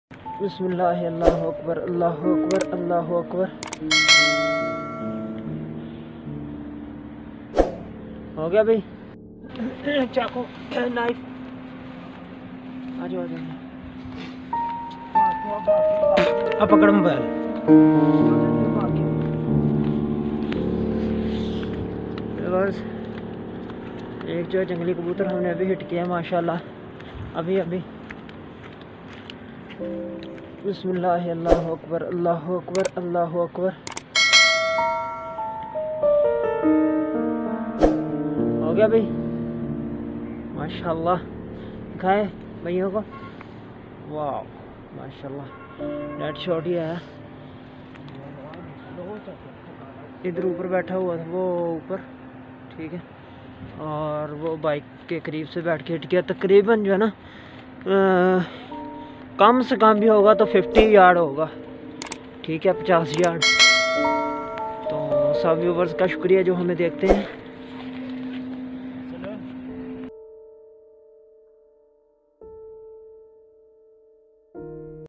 Amazing YouTube Feral Pigeon Hunting With Hatsan Airgun | Bird Shooting Live | Watch Full YT Episode
There is hunting of birds specially feral pigeon hunting
and shooting with hatsan air gun on amazing YouTube
hunting YouTube channel.
 I hope you enjoy my hunting videos on my
amazing YouTube hunting channel.
Hunting channel.